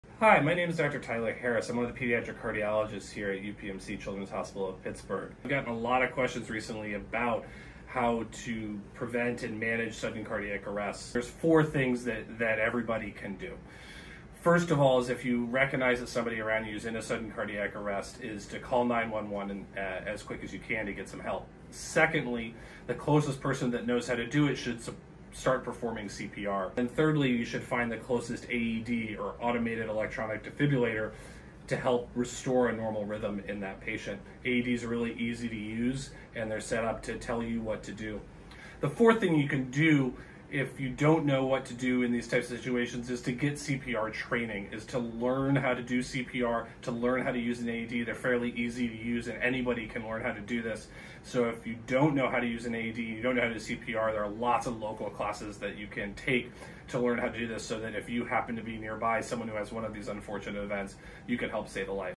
What To Do If Someone Is Experiencing Sudden Cardiac Arrest
Sudden cardiac arrest can happen to anyone, at any time. And when someone is in sudden cardiac arrest, it’s important to know what to do. With some basic first aid techniques, you can double or even triple a person’s chance of survival. Pediatric cardiologist Dr. Tyler Harris shares the 4 steps to take to save a life. ❤️